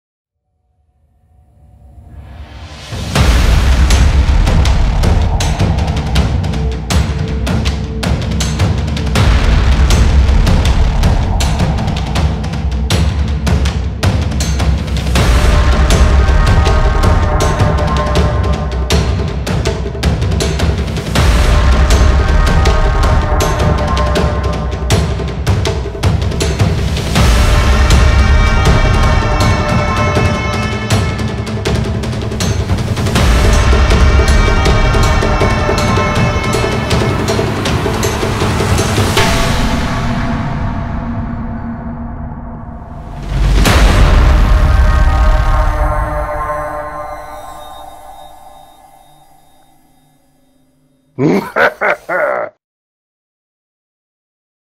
Iron Nookhaz ov da Horde
Artel "W" Miniatures presents
in association with ForFun Miniatures

ARRIVIN` OV DA IRON NOOKHAZ

Starring:
Dum-Dum Blitza-nookah
Smazhkila Ardmighta-nookah
Tah-Ya-Faze Witkunna-nookah

with additional appearance of
Sqoubbler Da Gobbo
and
Sgt. Underfoot

They say that Orctober is over, but it is not. Orctober rising again, in the tide of IRON!
THEY are on their way! Orctober 29th, 2020 - Iron Nookhaz at ArtelW.com! IRON AND RAGE !!!